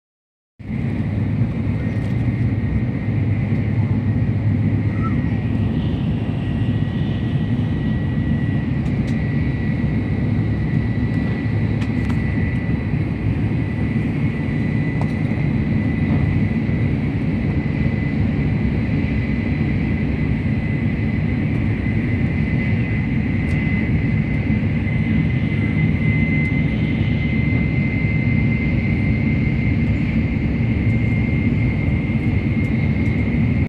Shinkansen Inside--feel the speed!!
Feel the speed inside a Shinkansen
A journey towards Heroshima from Tokyo